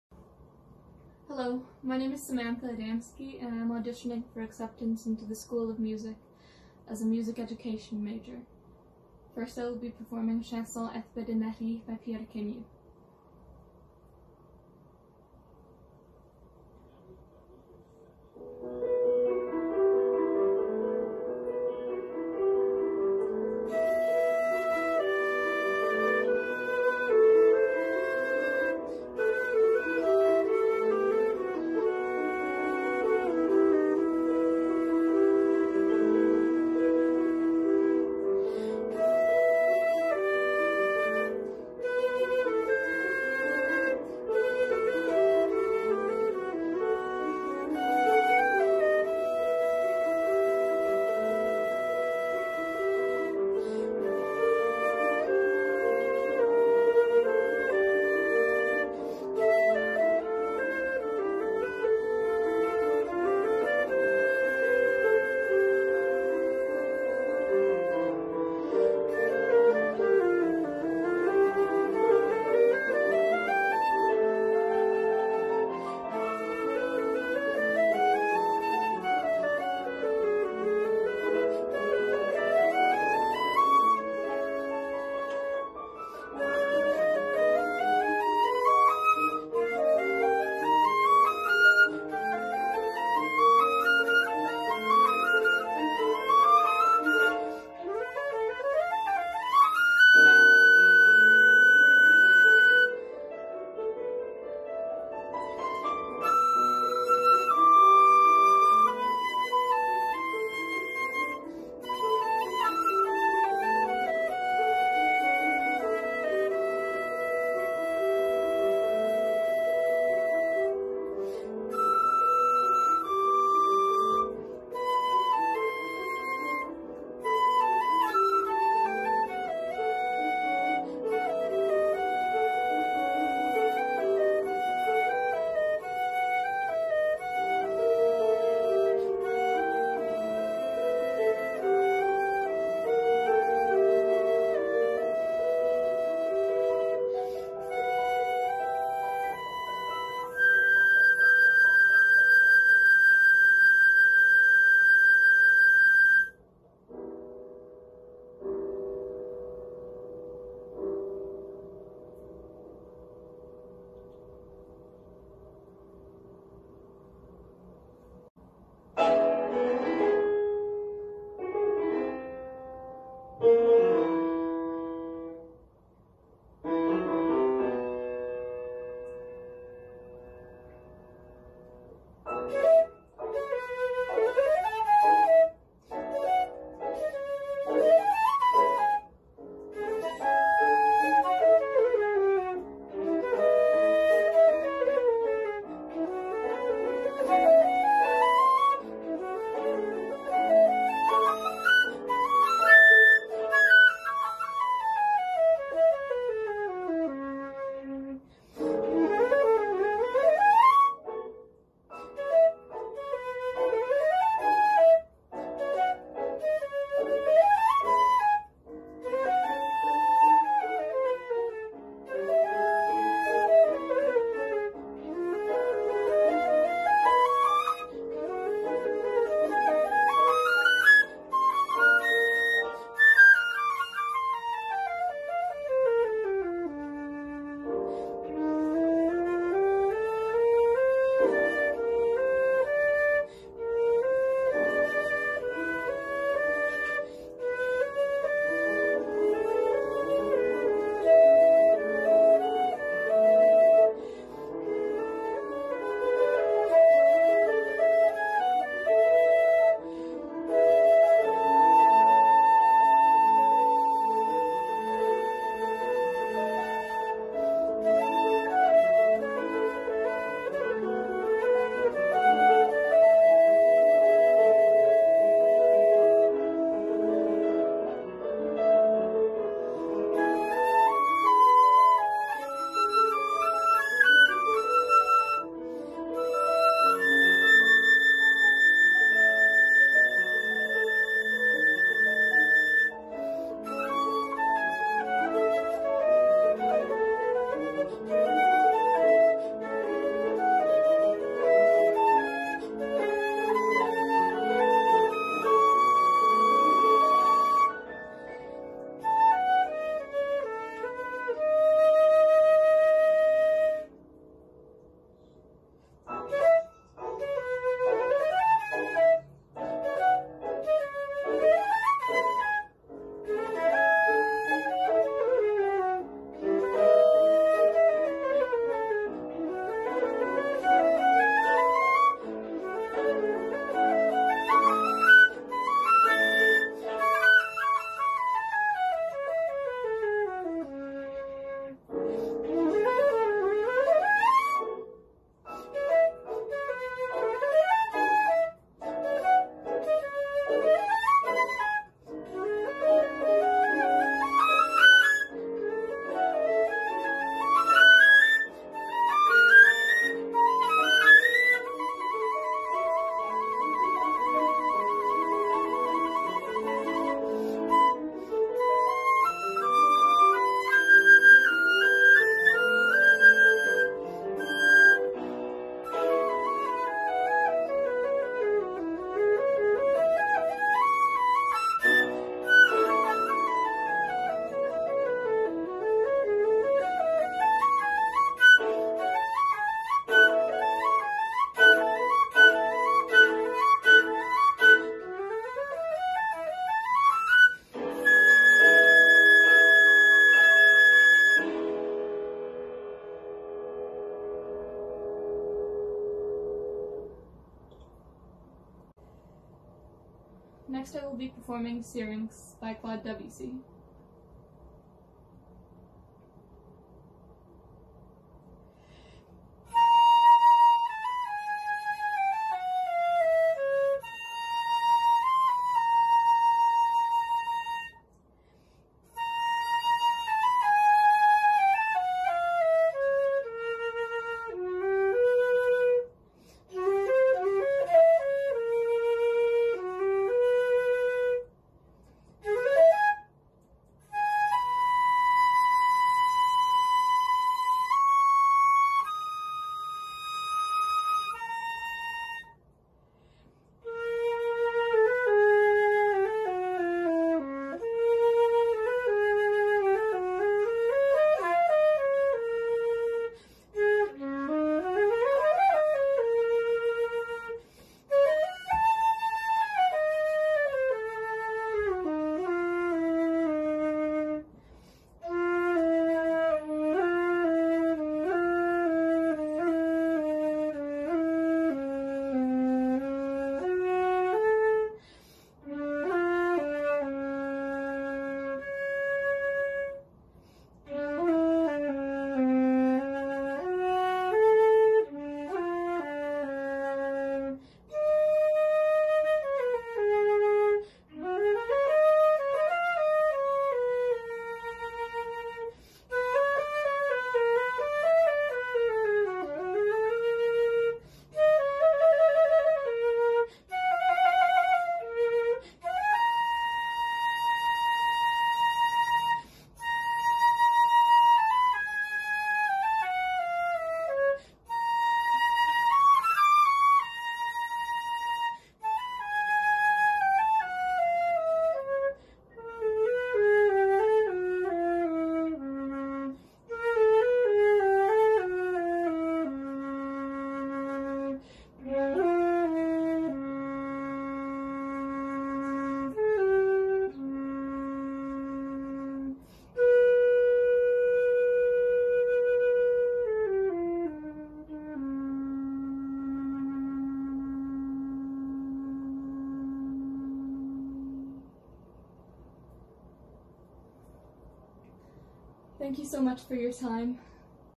woodwind jury: audition, Adamski
This video serves as my virtual audition for acceptance into the Ball State University school of music to be a music education major. It has been my dream for as long as I can remember to pursue this career path; to touch the lives of our youth as my band directors have touched mine. To the woodwind jury: thank you so much for your time and consideration.